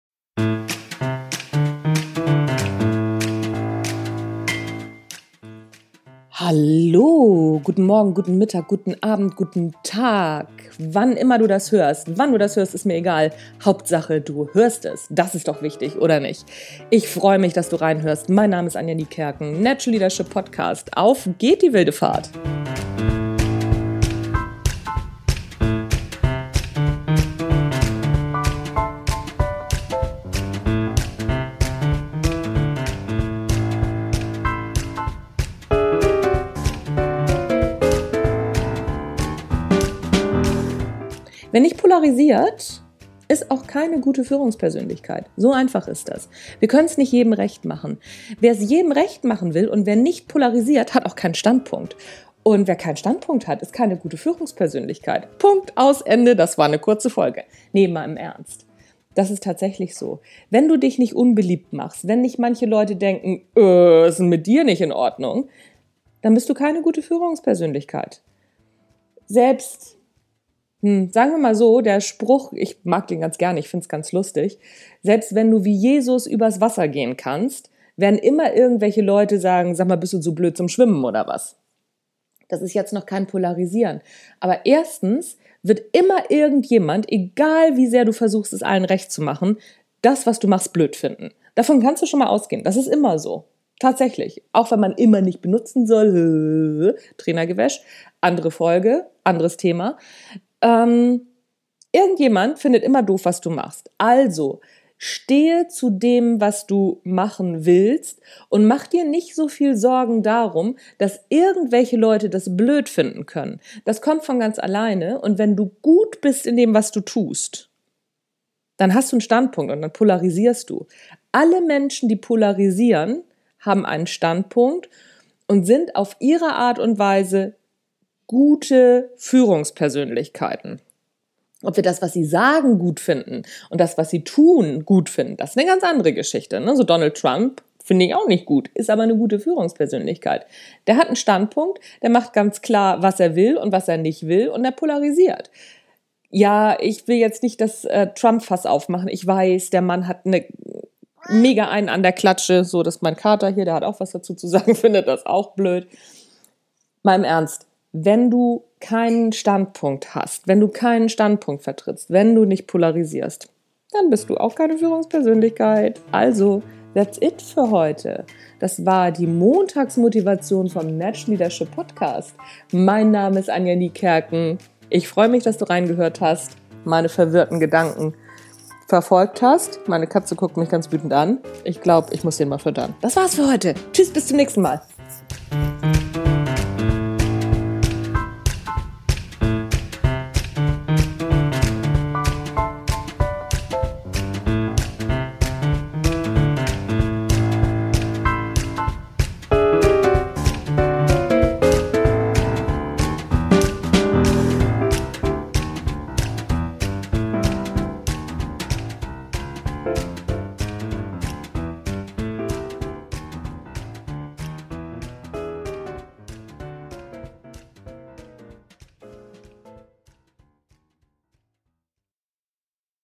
NL 410 – Montagsmotivation – Führen heißt polarisieren - Natural Leadership Podcast
Wer keinen klaren Standpunkt hat, kann nicht führen. Wer aber einen klaren Standpunkt hat, polarisiert auch ... Wer führen will, kann es nicht jedem Recht machen. Mehr dazu in dieser Folge.